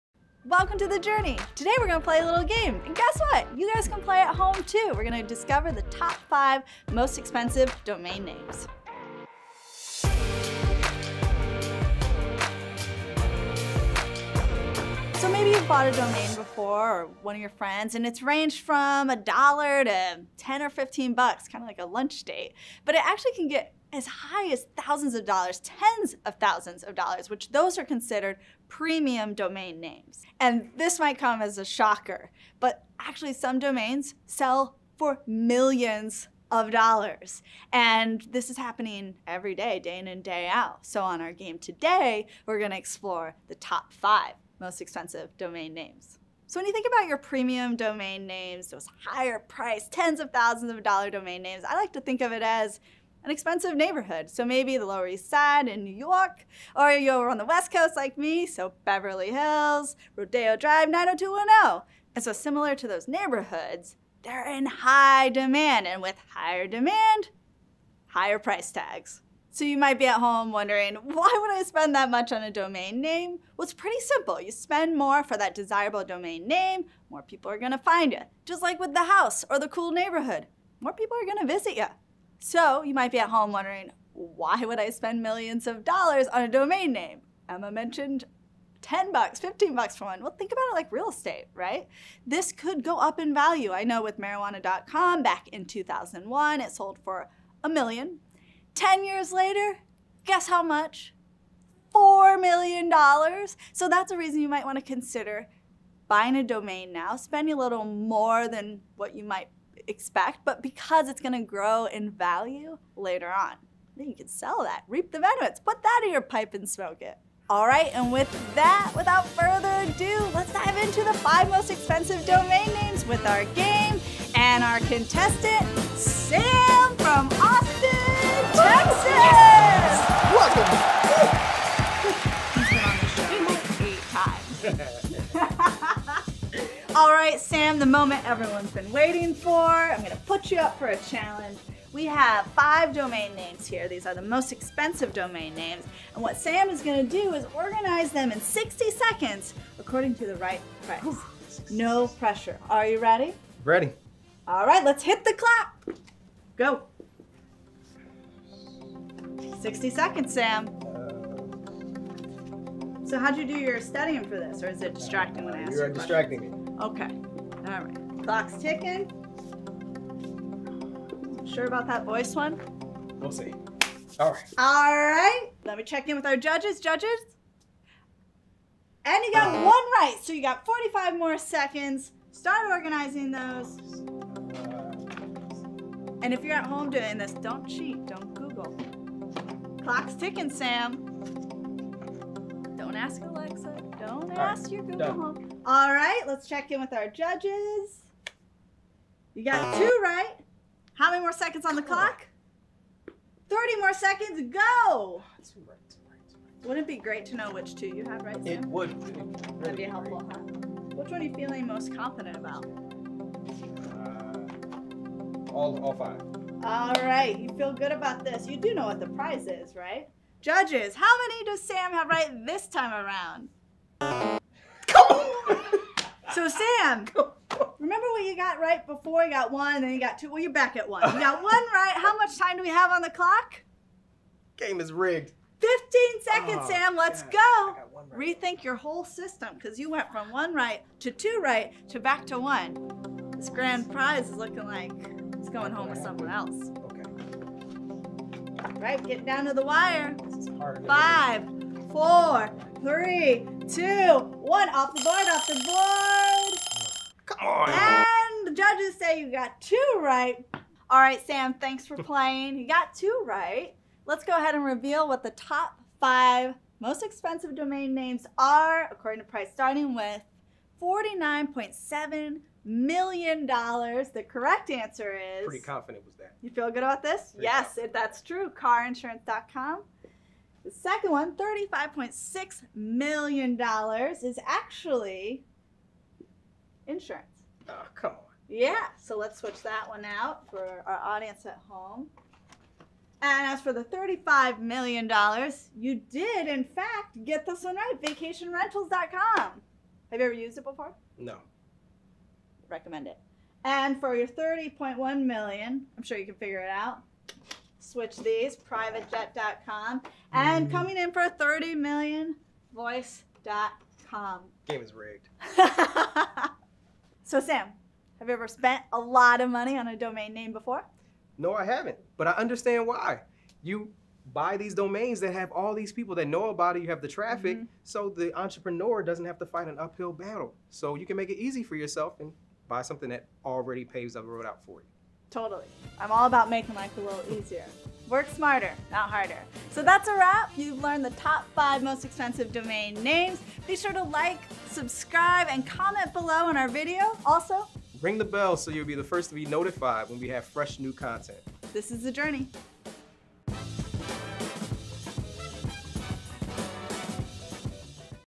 The Top 5 Most Expensive Domain Names l You won't believe the last domain
Welcome to our game show! Play along with the video before reading the answers below!

When most people think about the price of a domain, something in the $15 price range usually comes to mind. But often, domain names sell for much more than that. The average cost of buying a domain name that someone else owns on the secondary market is in the thousands of dollars.

It is not unusual to see public domain sales reported weekly in the tens or hundreds of thousands, and often enough there will be domain sales reported in the millions. Nailing down the most expensive domain names isn’t easy — most of the parties involved often try to keep that information private. There are various reasons for this practice, ranging from wanting to keep their competition in the dark to not wanting others to know how much they’re willing to spend on a domain in the future.

It’s a pretty common assertion that the vast majority of high-value domain sales are done with some sort of non-disclosure agreement in place. However, enough sales are public, so we can still get a good idea of the domain market and its health over the years.

The most expensive domain name ever sold came with a hefty price tag of $872 million. Yes, you read that right. Cars.com was valued at $872 million (we get that number from the SEC filing, courtesy of the parent company, Gannet Co., Inc.). Imagine the look on someone’s face when they were told their domain was worth that much!

Many people don’t report the sale of their domains publicly, so “most expensive domains” lists vary. But as a whole, most people are in agreement with most of the highest sales reported for domain names. Coming in at the number two spot is Insurance.com selling for $35.6 million, followed by VacationRentals.com at $35 million, then PrivateJet.com at $30.18 million, and Voice.com at $30 million.

So, why are people willing to pay so much for a domain name? In the current decade, a brick-and-mortar address isn’t quite as important as it once was for many businesses. In many cases, it’s worth spending more to get a premium domain name — one that is short, catchy, and easy to remember.

Many premium domains end with .com, but they can use many of the newer domain extensions like .business, .guru, .app, .store, and others. Adding dashes and creative spelling can give you more options, but those strategies also confuse potential customers. That’s why companies usually try to buy the domain name that exactly matches their business name.